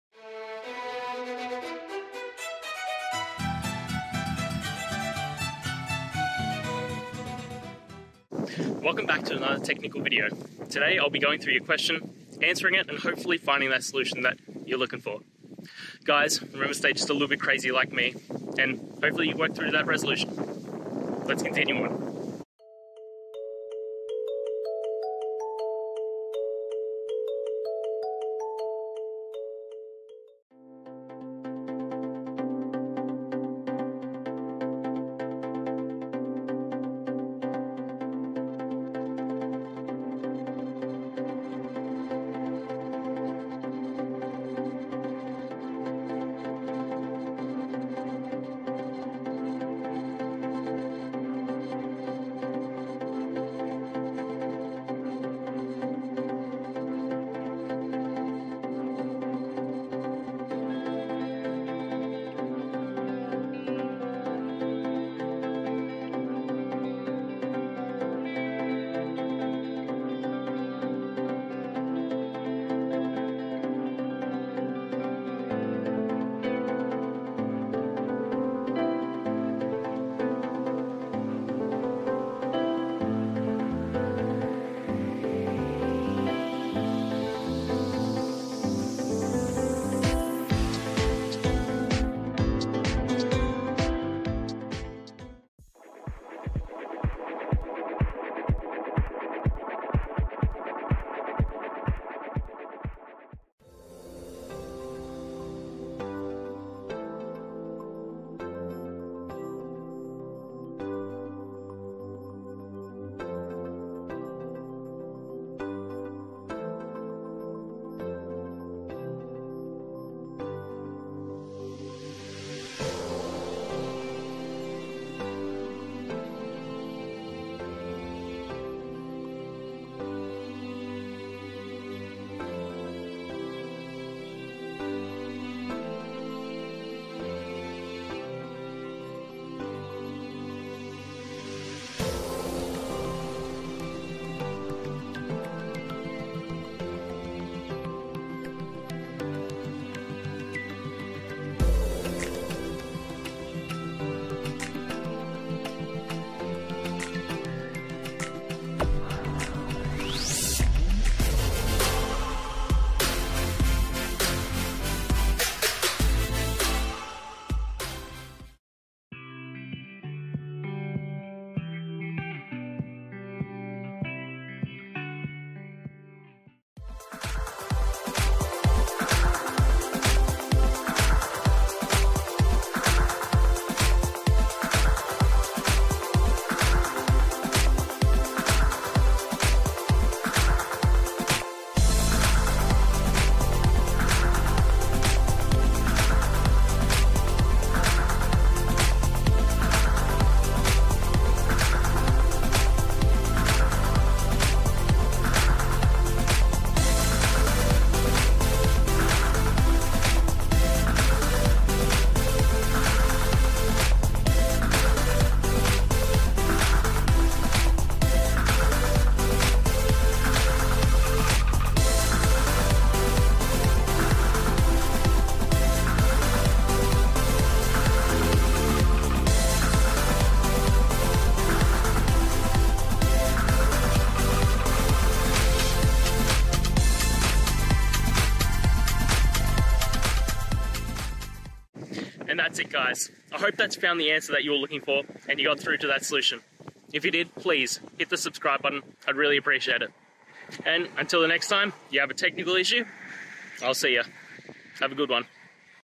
Fixing Import Errors with pandas.plotting: Common Issues and Solutions
In this video, we dive into the common import errors encountered when using the pandas.plotting module in Python. Whether you're a beginner or an experienced data analyst, understanding these issues is crucial for seamless data visualization. Join us as we explore practical solutions to help you troubleshoot and fix these errors, ensuring your plotting experience is smooth and efficient.

Today's Topic: Fixing Import Errors with pandas.plotting: Common Issues and Solutions

Related to